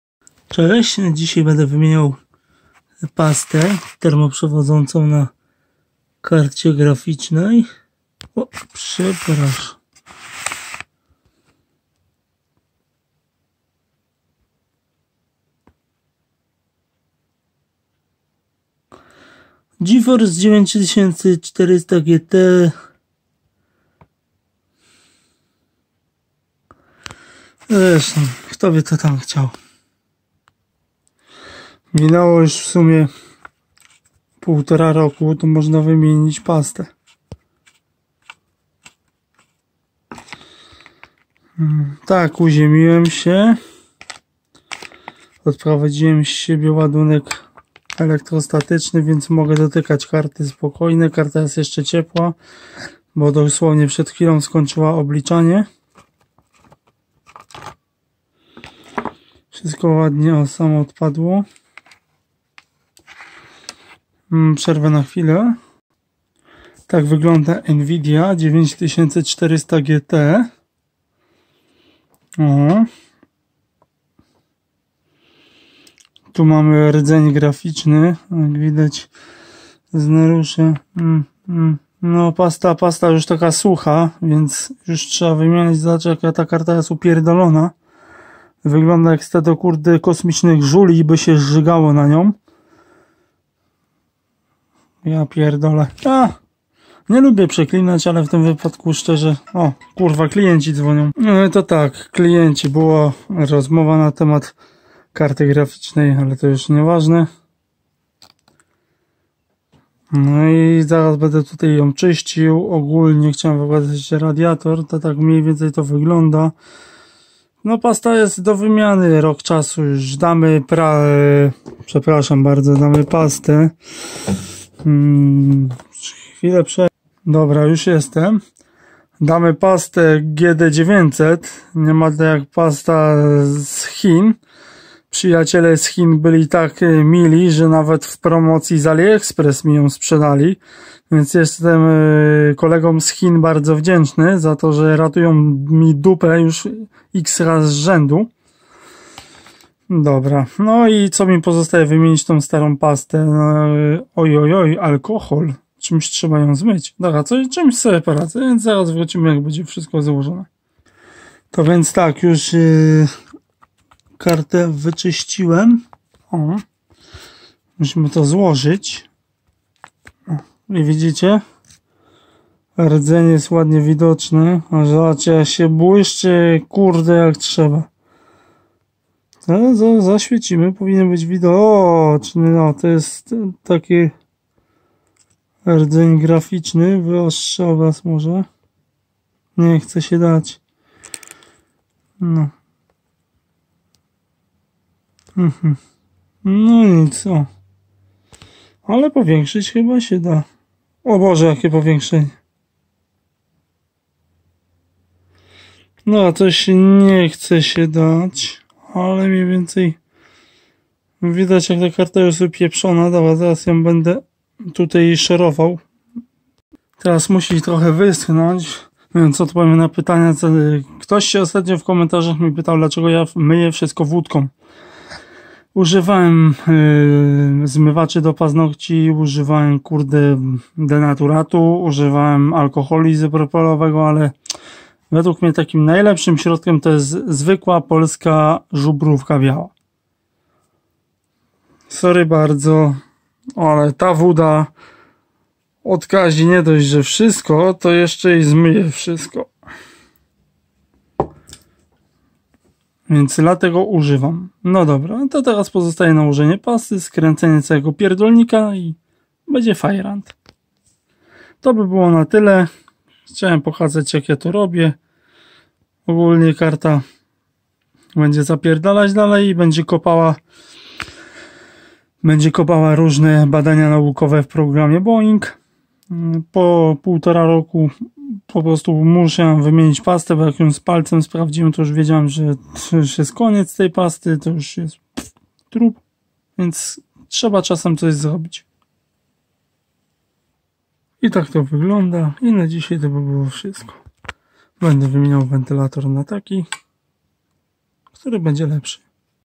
Nvidia GEFORCE 9400GT
Modernizacja karty graficznej do programu Boinc. Obliczenia dla Nauki